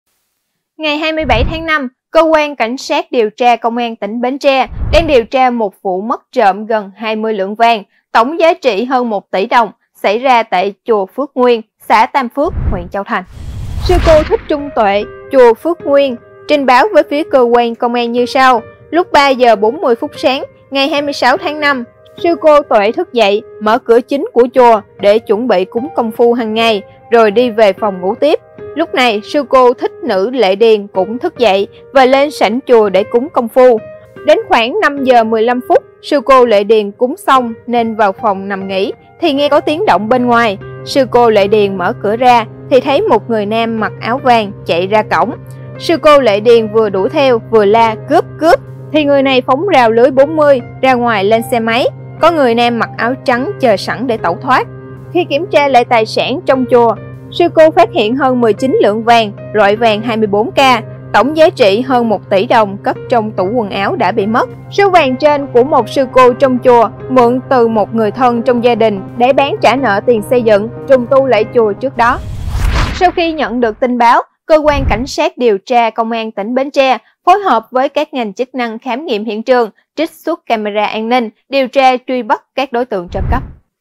Bến Tre - Nhà chùa bị mất gần 20 lượng vàng
Ngày 27/5, Cơ quan CSĐT Công an tỉnh Bến Tre đang điều tra một mất gần 20 lượng vàng, tổng trị giá hơn 1 tỉ đồng xảy ra tại chùa Phước Nguyên, xã Tam Phước, huyện Châu Thành.